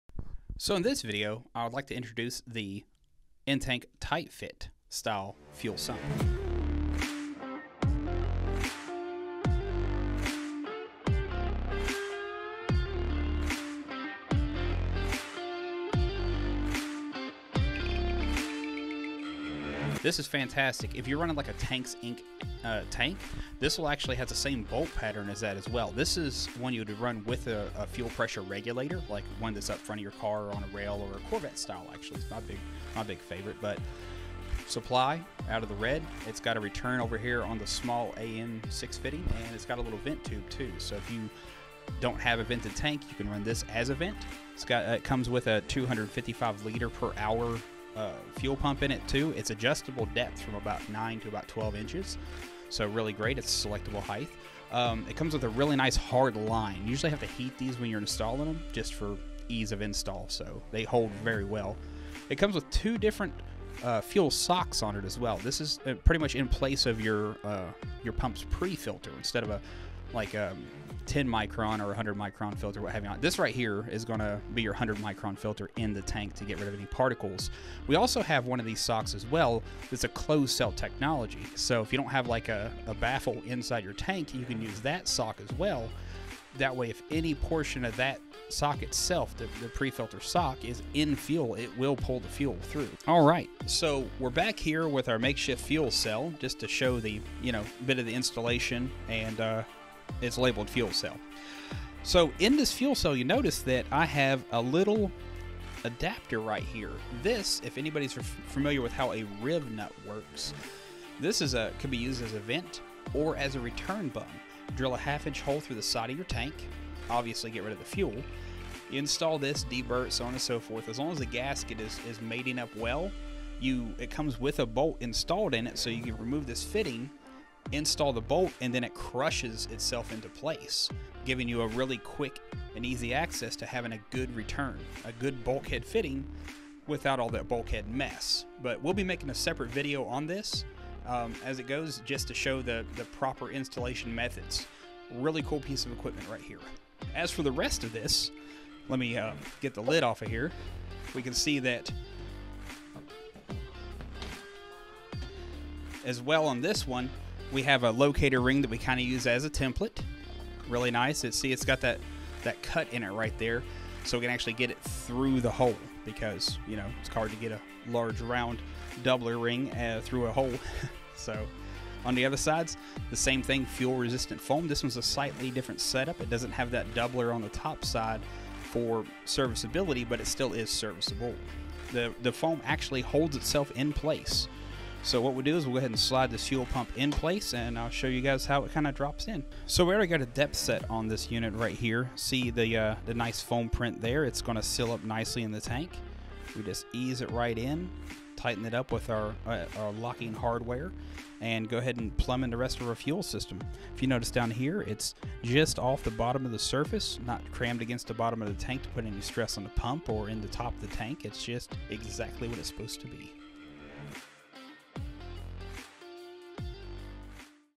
Tight-Fit In-Tank Fuel System
The Tight-Fit In-Tank fuel pumps are a fantastic option for switching to EFI! Today, Tim walks you through some of the key features of this new systems, and how to drop it into the tank. Head over to acesefi.com for more information or to pick one up for yourself!